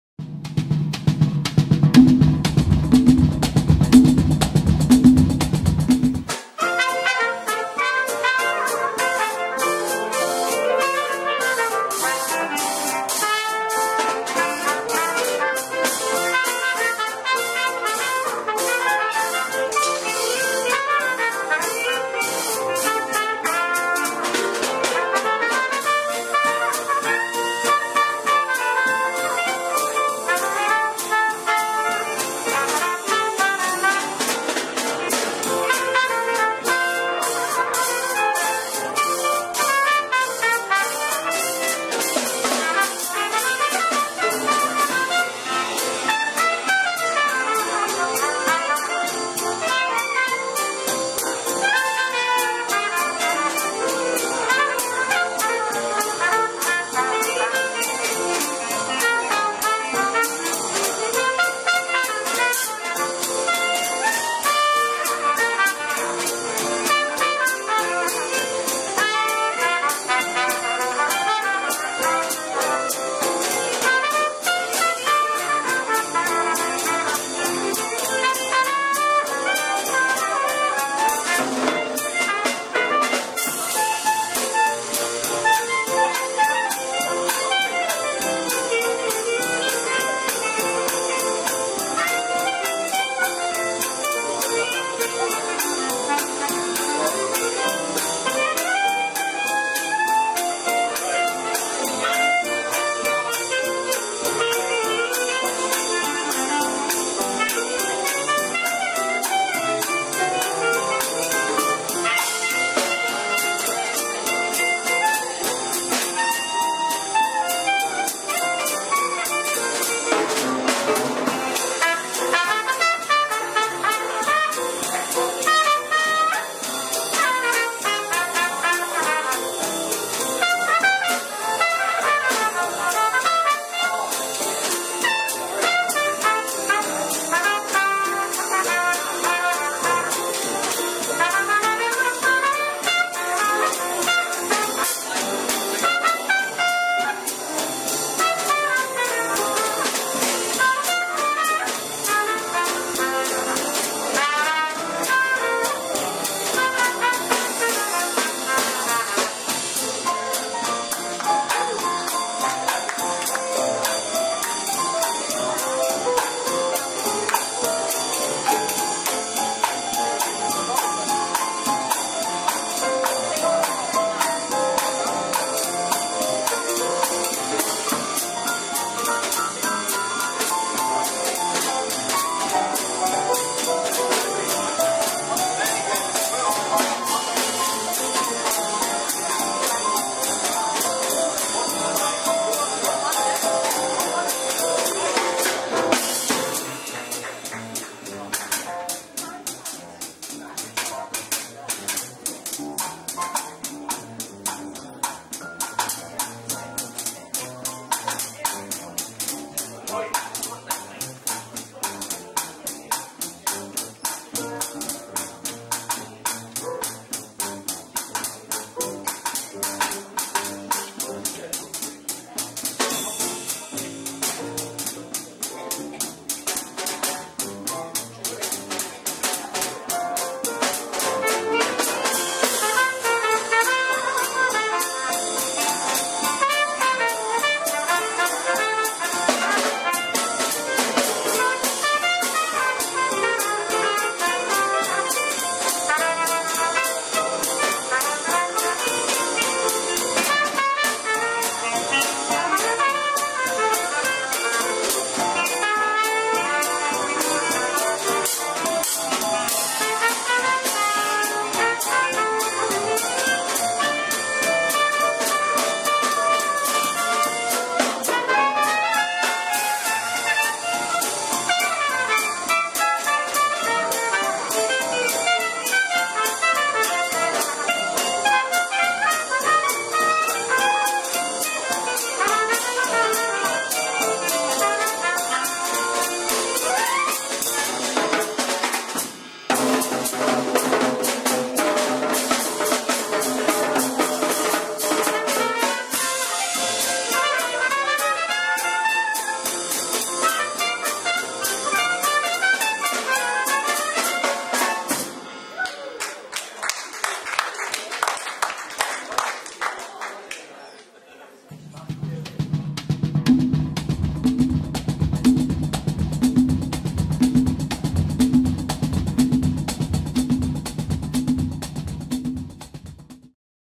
SAJA Major Exhibition 2016
Invitation for South Australian Jazz Archive Major Exhibition "The Story of Jazz in South Australia" as part of South Australia's History Festival 2016